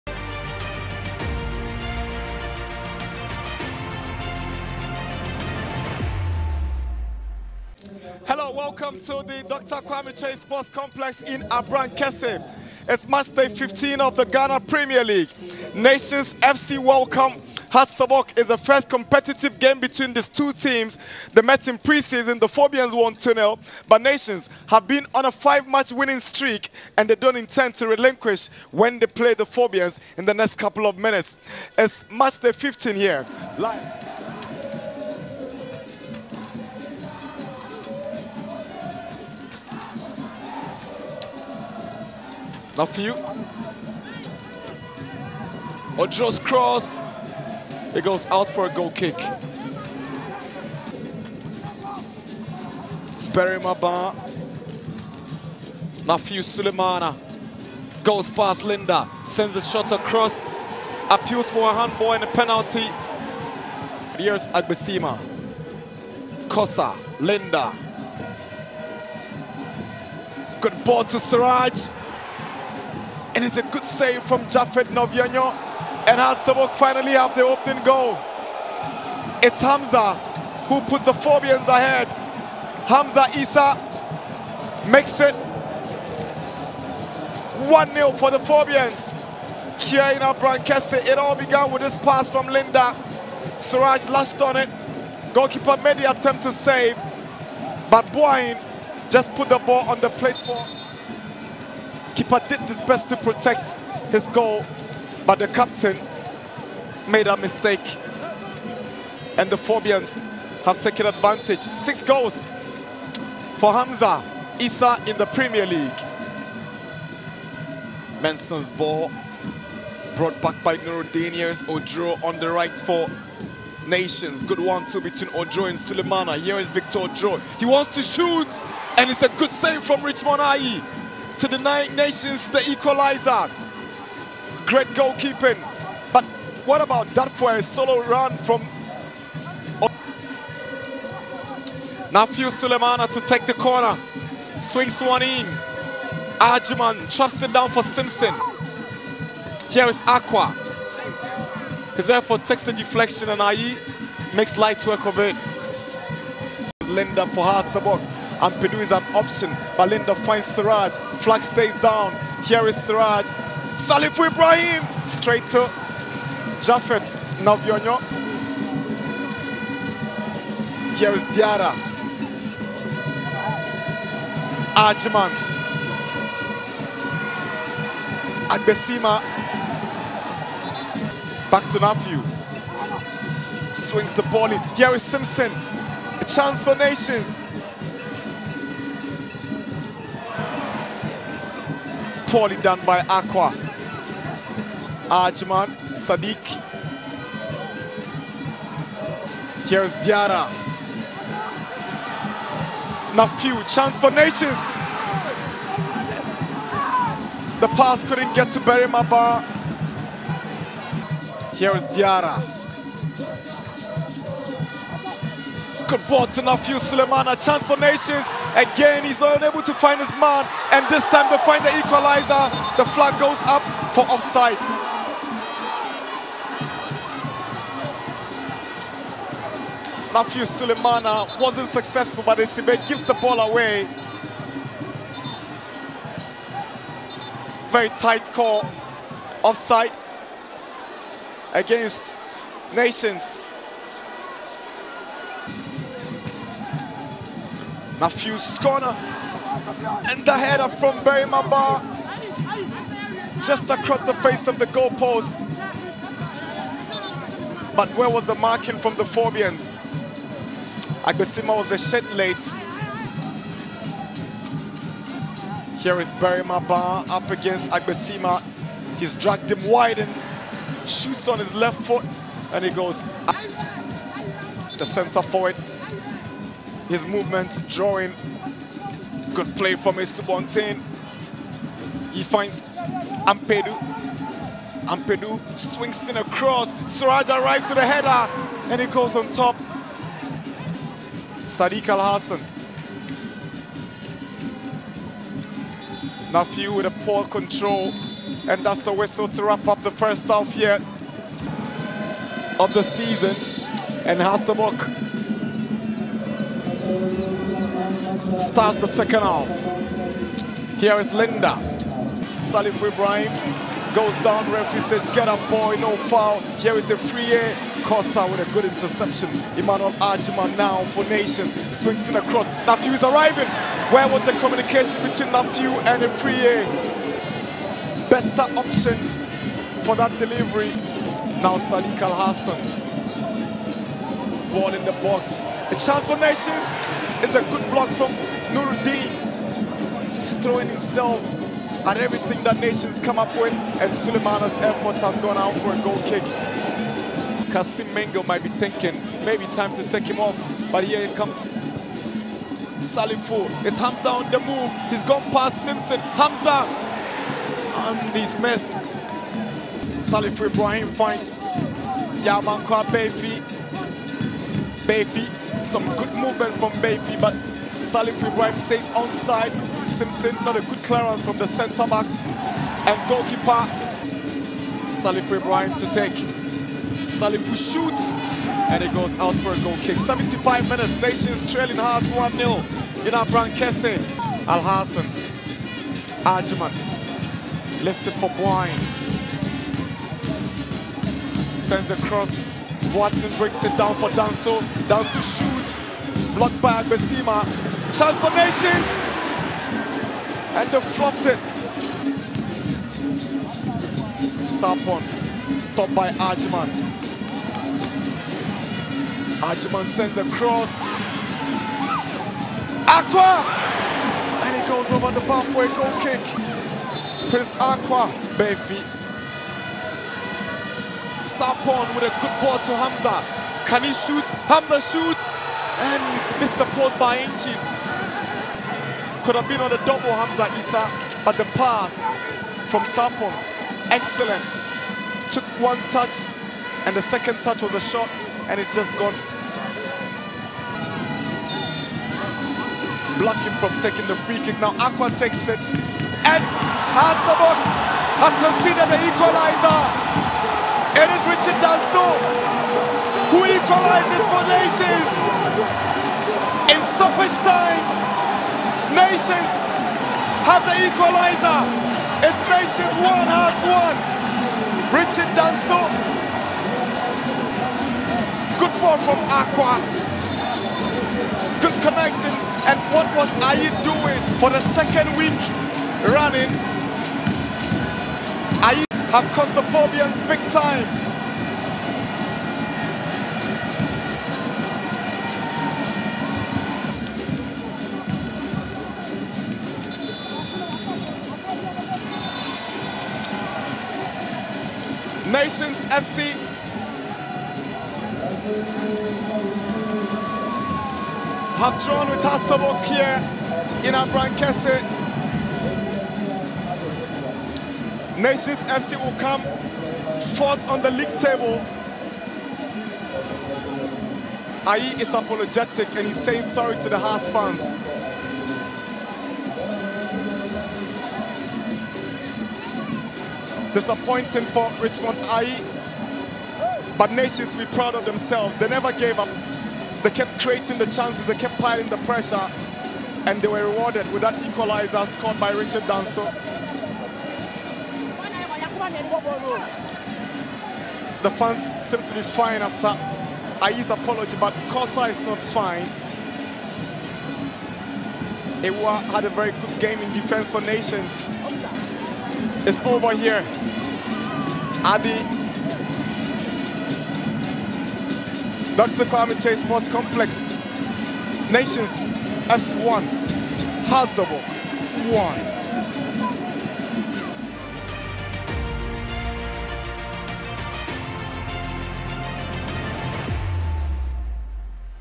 Nations Fc 1-1 Accra Hearts of Oak | Highlights | Ghana Premier League
Accra Hearts of Oak once again failed to secure three points against Nations FC, following their recent loss to the Porcupine Warriors of Kumasi in the Super Clash fixture of GPL Matchday 14. Hearts took an early lead with a goal by Hamza, but Richard Danso salvaged a draw for Nations FC with a 90+4' strike that beat goalkeeper Ayi and hit the post, concluding the match in a 1-1 draw.